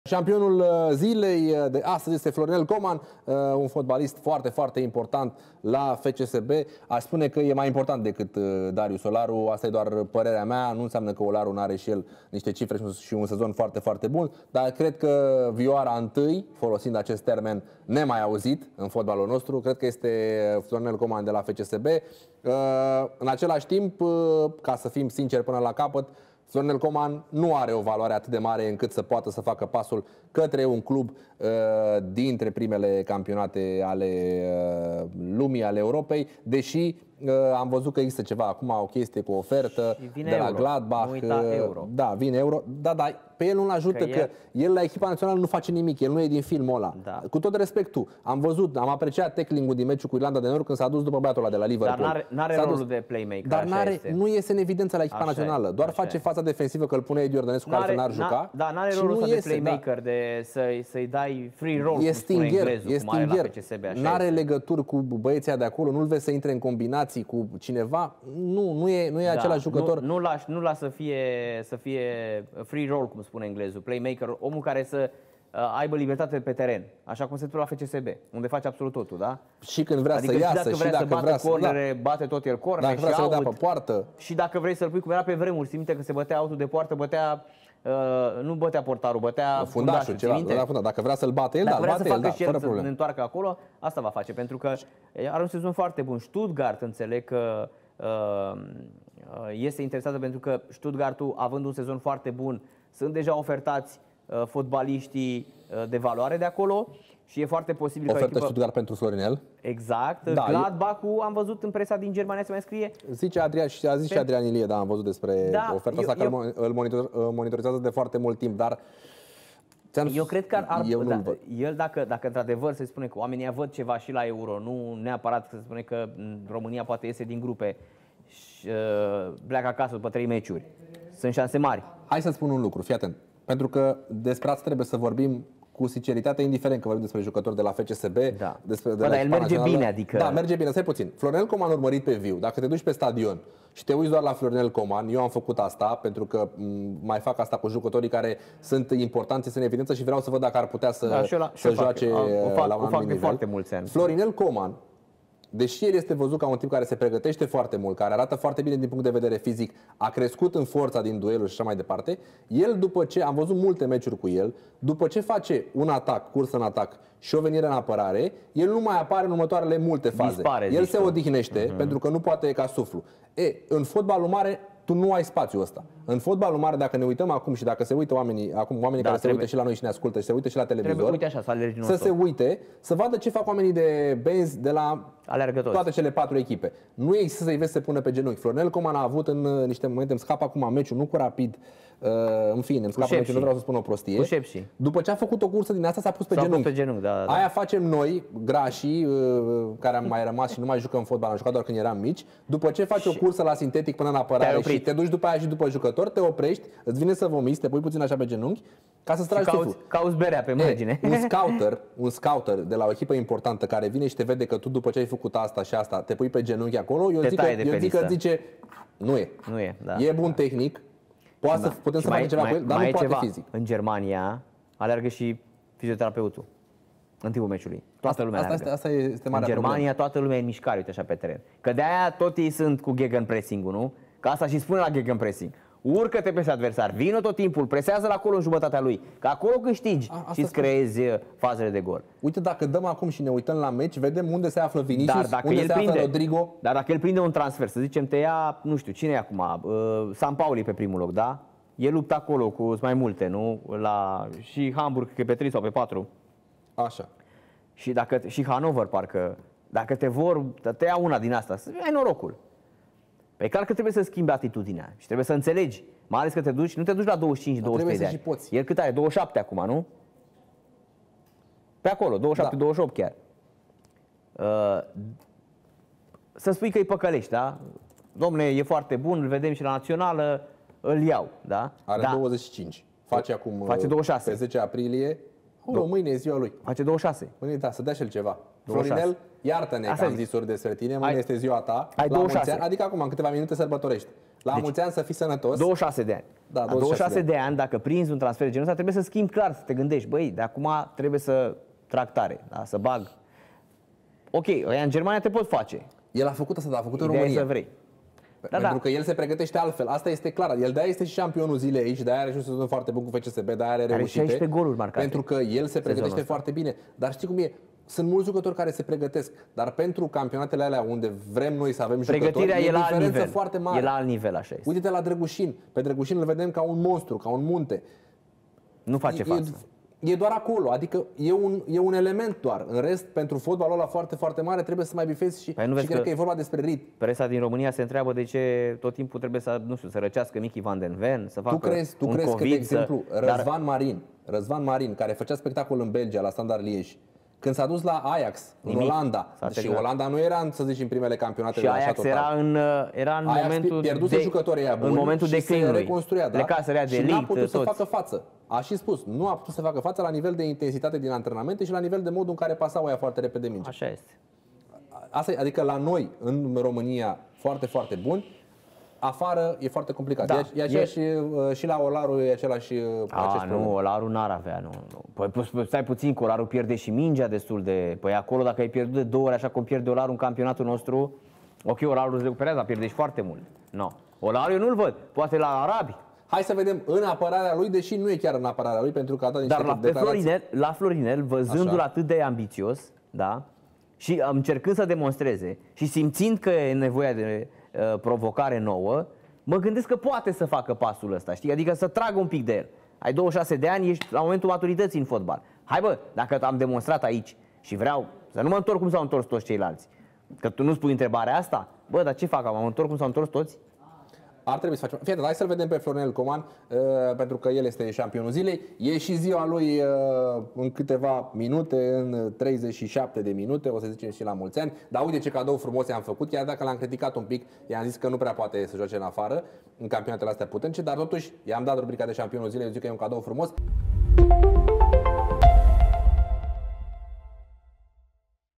„Florinel Coman nu iese în evidență la prima reprezentativă, nu va ajunge la un club de top”
Alberto Boțoghină și Narcis Drejan vor dezbate meciurile europene pe rețelele sociale și pe site, într-o ediție de peste două ore, inclusiv în timpul duelurilor din UCL. În premieră, emisiunea Gazetei Sporturilor va fi difuzată audio, în toată țară, la Radio Sport Total FM.

euro gsp,gazeta sporturilor,gsp.ro,superliga,liga 1,cfr,dan petrescu